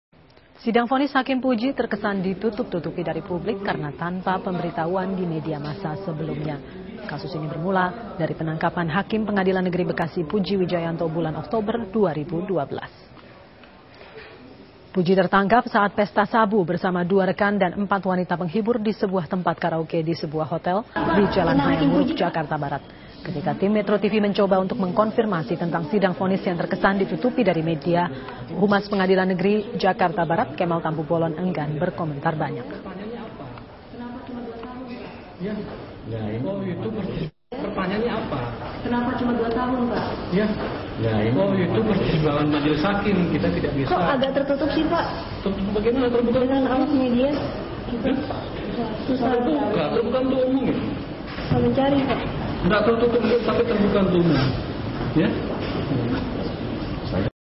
Hakim Puji Wijayanto Divonis Dua Tahun
Jakarta: Puji Wijayanto, hakim di Pengadilan Negeri Bekasi yang terjerat kasus narkoba, divonis dua tahun penjara. Celakanya, sidang vonis Puji di PN Jakarta Barat, tak pernah terendus publik.

Ada kesan, sidang vonis Puji sengaja diselimuti tabir. Pihak pengadilan seperti sengaja menutupi kasus ini agar tak menyebar ke mana-mana.

Humas PN Jakbar Kemal Tampubolon membantah tudingan. Ia memastikan tak ada yang ditutup-tutupi dalam sidang vonis Puji. "Tertutup bagaimana...? [Sidang] Terbuka untuk umum kok," kata Kemal.

Puji dicokok medio Oktober 2012, di tempat bernyanyi di sebuah hotel di Jalan Hayam Wuruk, Jakarta Barat. Ia tertangkap basah pesta sabu bersama dua teman dan empat wanita penghibur.